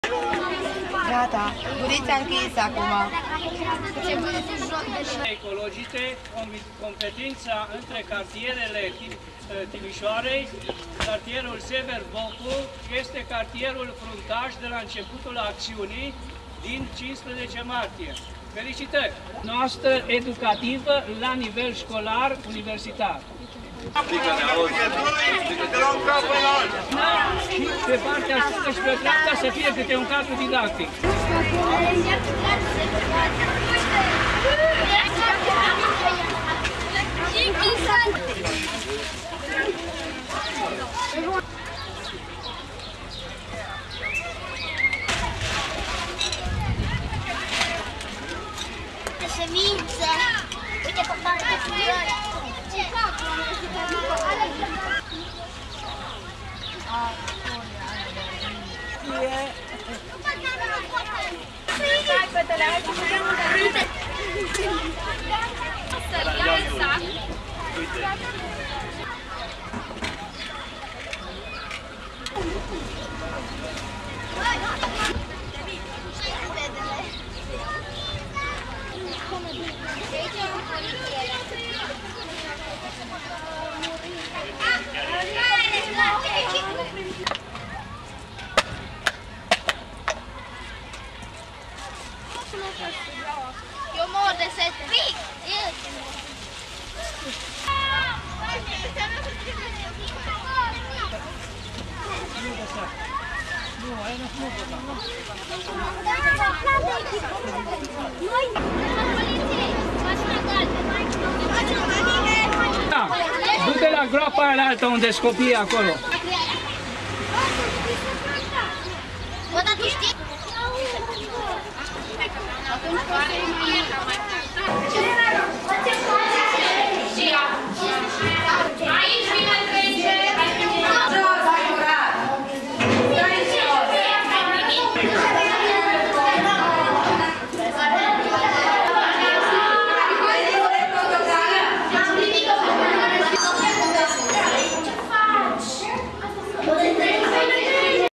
Ecological initiative in Timisoara
In Timisoara the grad school nr.7 and many other volunteers take care of the street cleaning and tree planting in Sever Bocu neighborhood